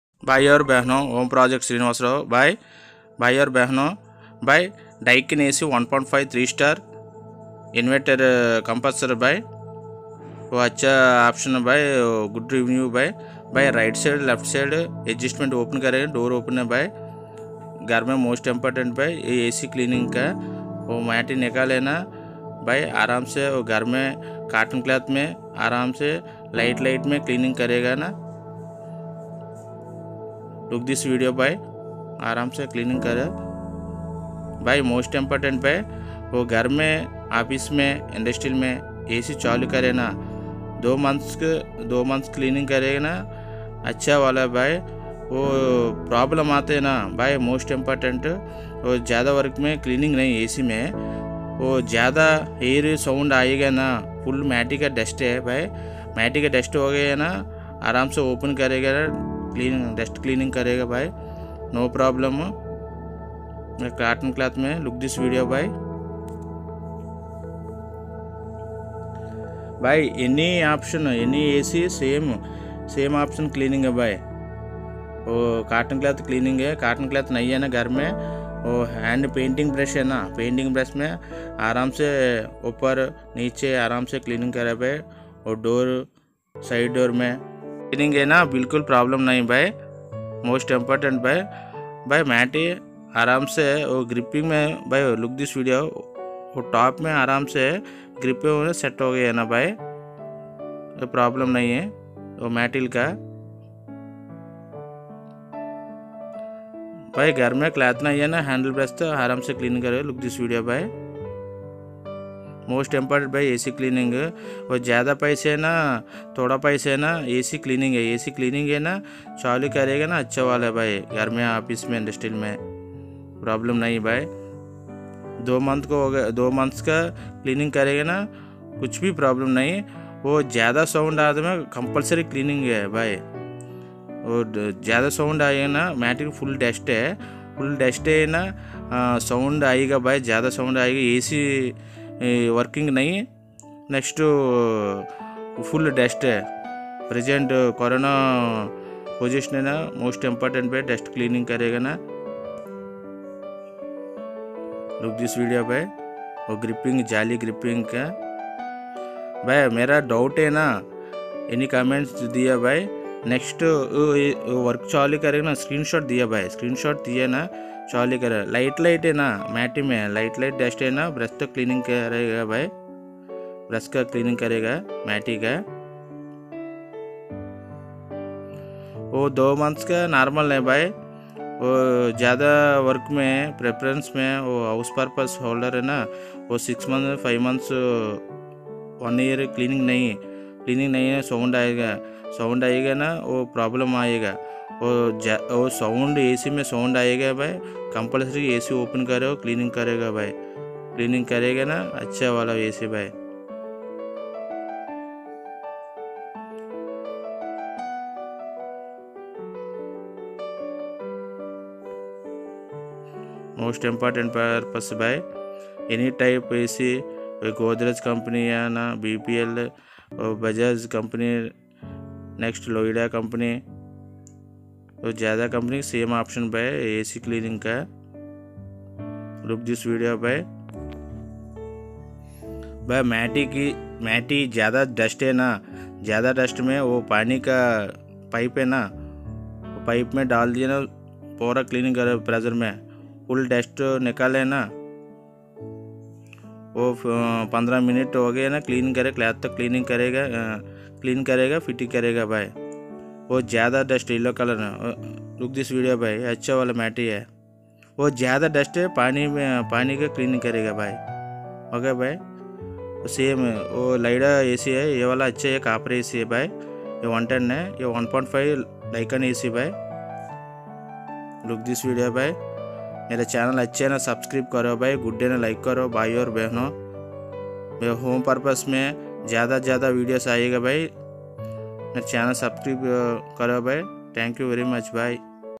AC cleaning and servicing Daikin AC 1.5 Ton 3 Star inverter option AC cleaning and servicing |Hindi
electrical_plumbing_wiring_vasu Hi friends this is AC cleaning Daikin AC 1.5 Ton 3 Star inverter AC cleaning above 15 years gent's and ledes parctical me